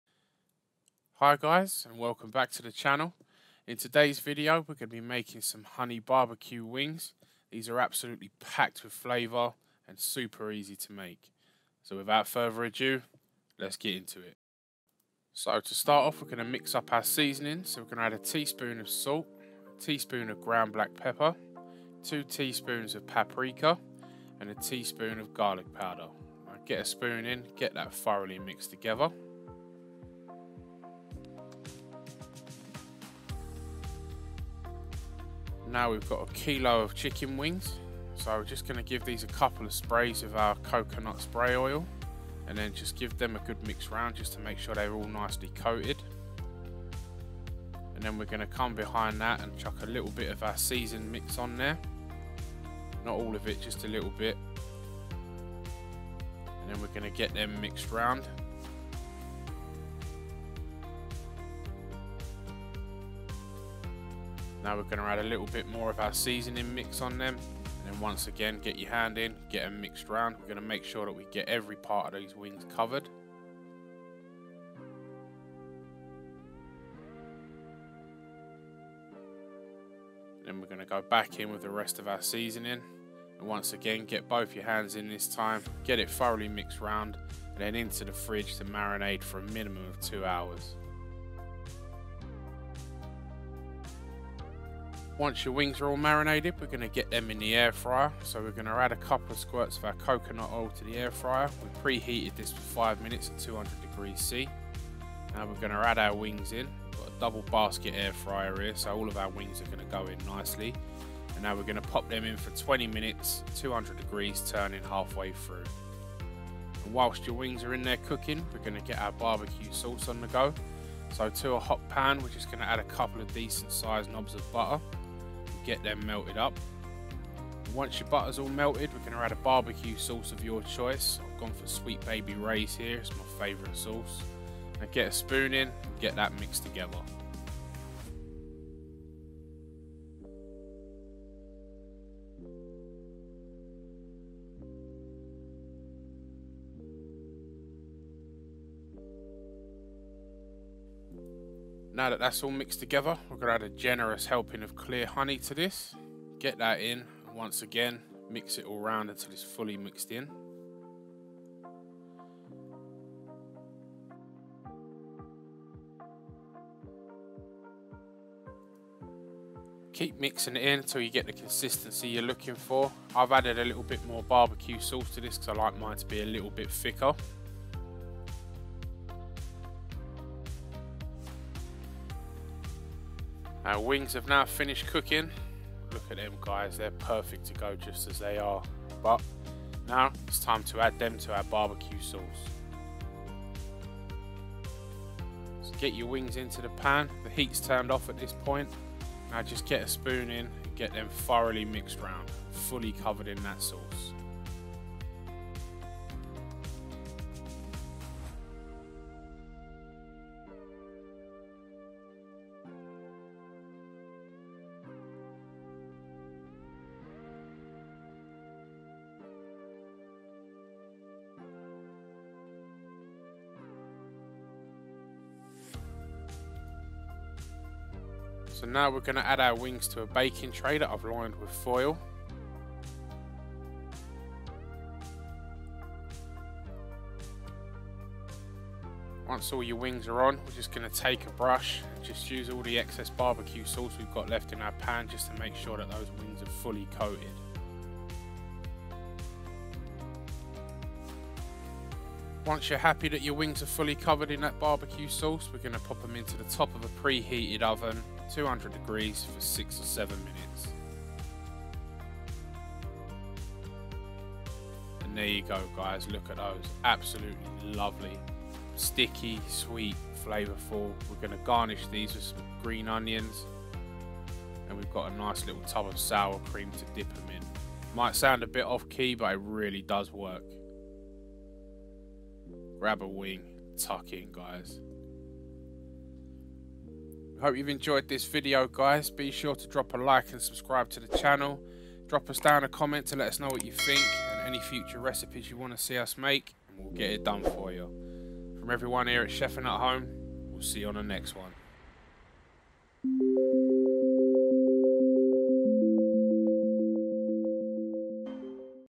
Honey BBQ Wings | The Best Honey BBQ Wings Recipe
We have another banging, easy yet delicious recipe for you today Honey BBQ Chicken Wings, These are super easy to make and so tasty! They do down perfectly with an ice-cold beer or soft drink, try dipping them in some sour cream & chive dip… It really works well. Please enjoy!

Ingredients:

Ingredients:

1kg Chicken Wings

Seasoning:
1 tsp Ground Black Pepper
1 tsp Garlic Powder
1 tsp Paprika
1 tsp Salt

Honey BBQ Sauce:
2 nobs of butter
1 ½ Cups of BBQ Sauce
2-3 tbsp Clear Honey